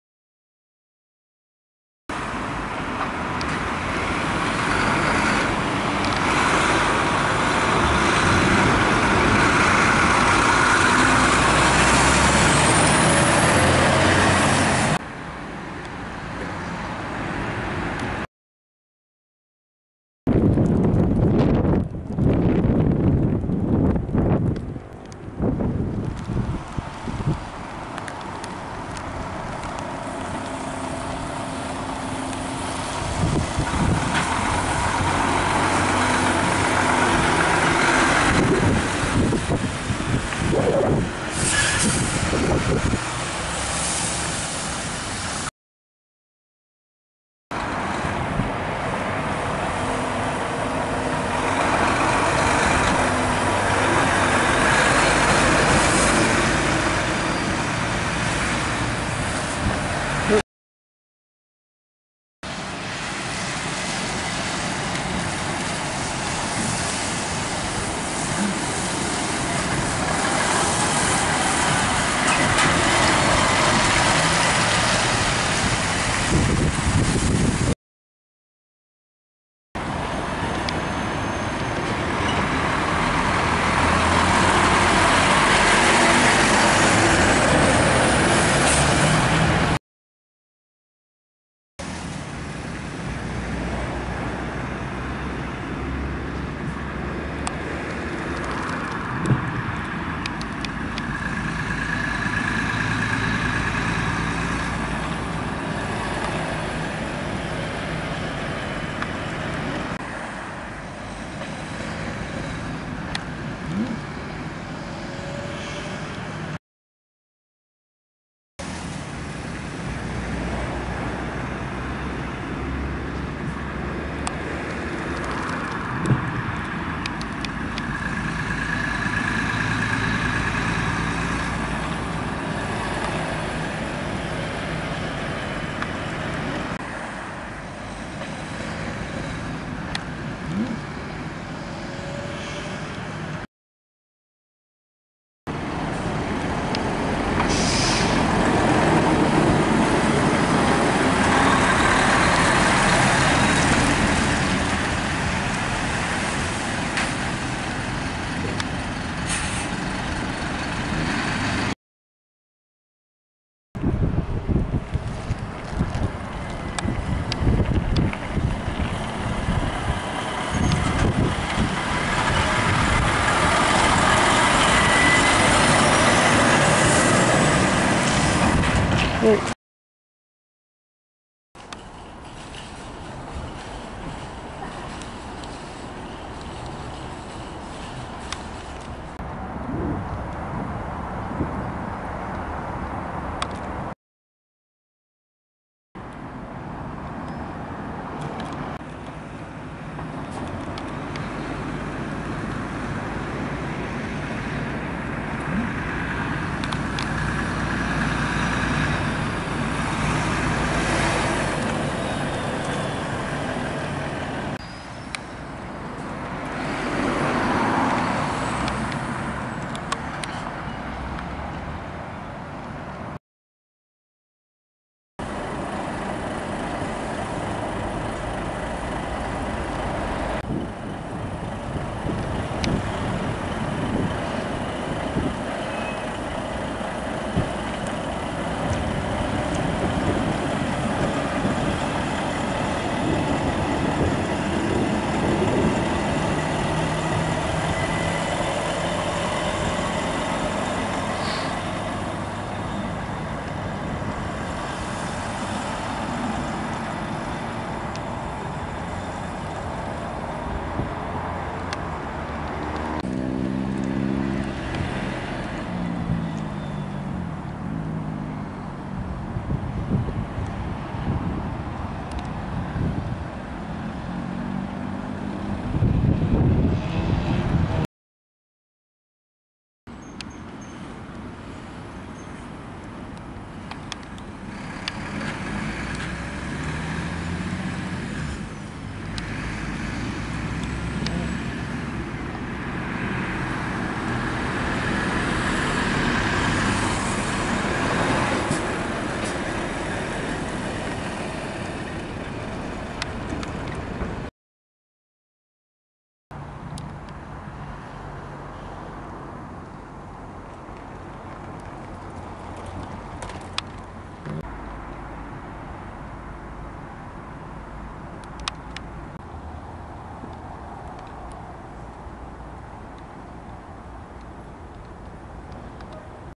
EDDIE STOBARTS AT TESCO BROCKWORTH FEB-MAR 2016
Eddie Stobarts with  Double Deckers at Tesco Brockworh,Gloucester over a variety of nights